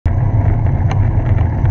Steenstraat fietspad werken 6
als je na 4 weken en 8 meldingen nog altijd zelfde situatie aantreft is het niet verwonderlijk dat je geen enkel vertrouwen of respect meer overhoudt voor overheidsdiensten